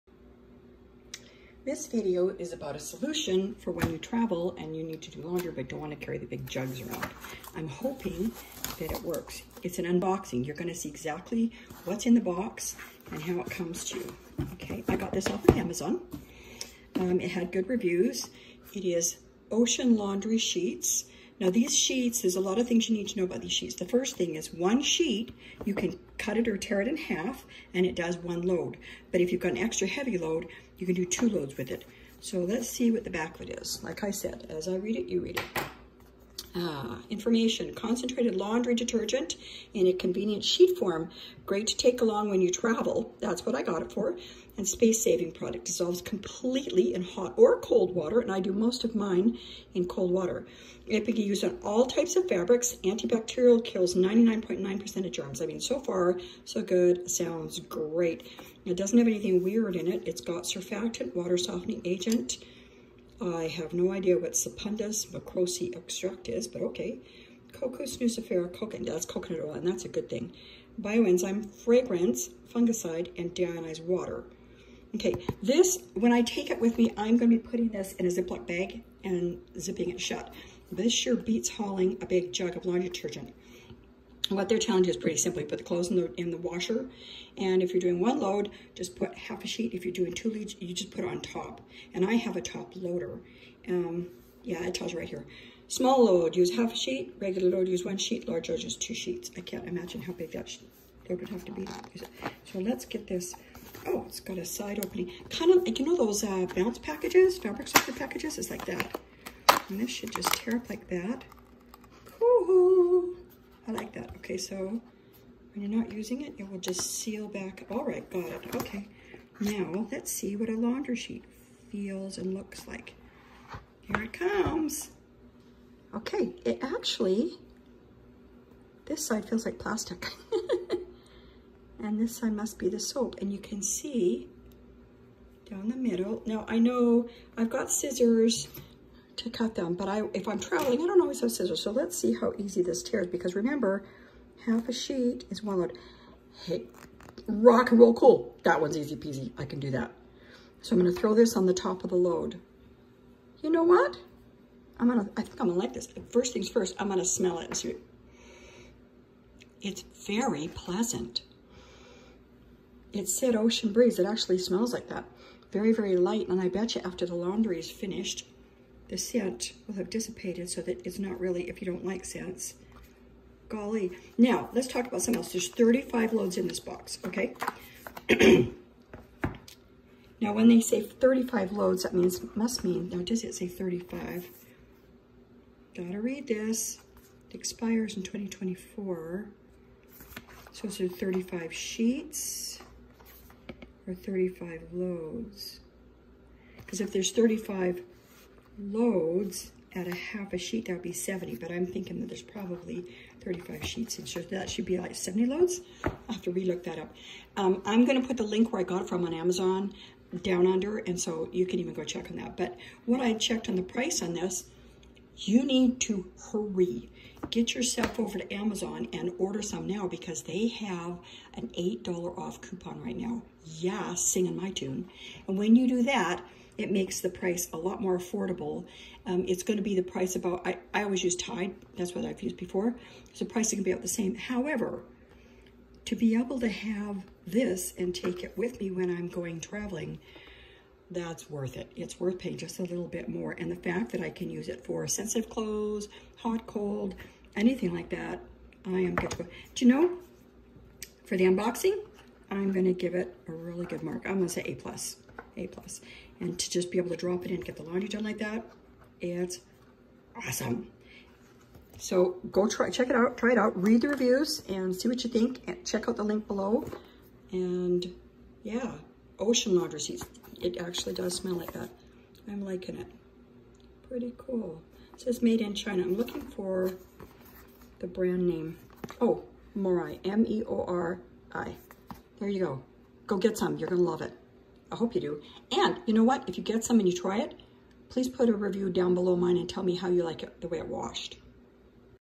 Laundry sheets by Morei - unboxing video.
These laundry sheets smell amazing and are so convenient to tuck in your suitcase pocket when you are traveling. No need to drag around heavy liquid laundry bottles.  Use a half sheet for small loads, one full sheet for regular loads, and 2 sheets for large loads.  They come in a fabric softener type box for convenient access.  Hurry and click on the link to go to their listing on Amazon as they have an $8 off coupon right now! Yeah! You don't want to miss that!

Update: Be aware that the sheets don't always completely dissolve.  Most of the time, they do, but in the last batch of clothing the sheet didn't and left a big glob of gooey blue undissolved substance from the sheet on the top of the side of the washer.  I had to run the washer again to get rid of it.